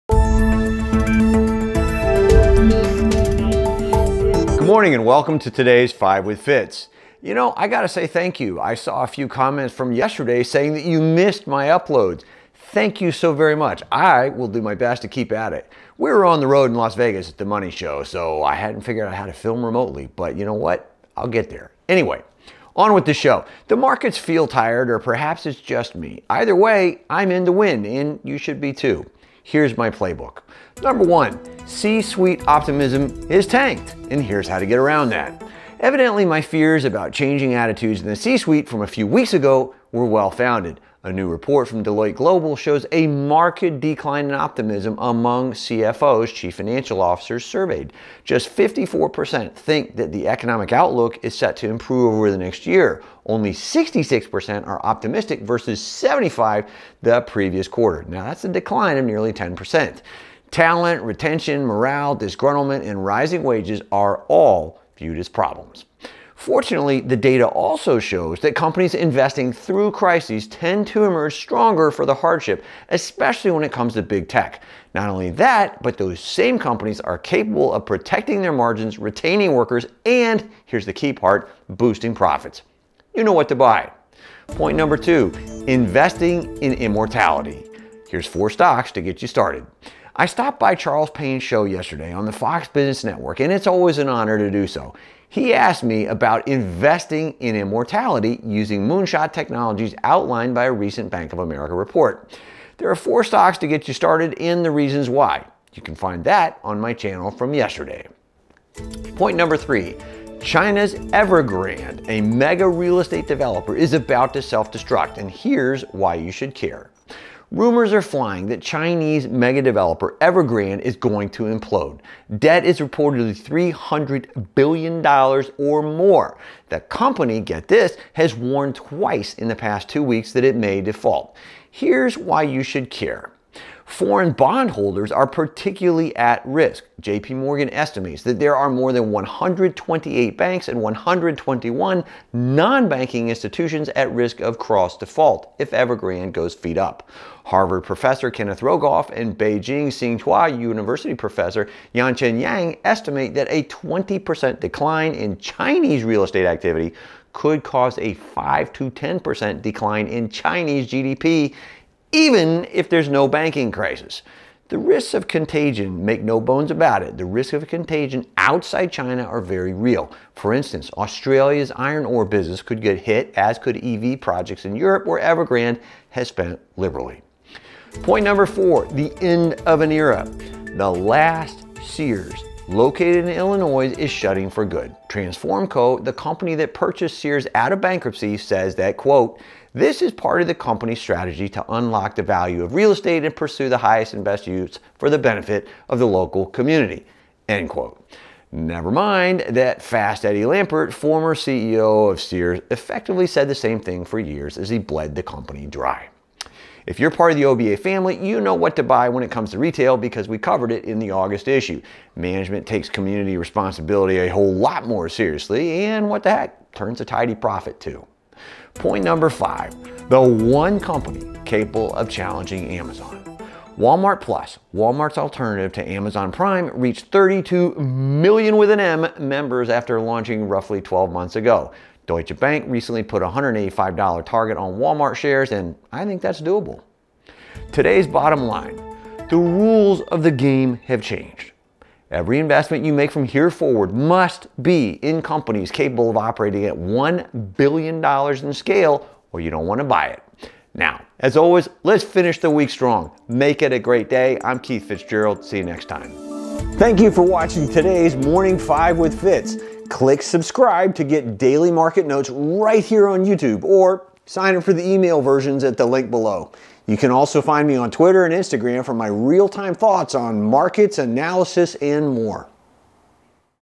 📝 Investing in Immortality?! | Daily Market Notes by Keith Fitz-Gerald
Invest like everybody else? No thanks. On today’s Morning! 5:

1 - C-Suite optimism tanks; here’s how to get around that
2 - Investing in immortality – 4 stocks to get you started
3 - China’s Evergrande is about to self-destruct – why you should care
4 - The end of an era
5 - The one company capable of challenging Amazon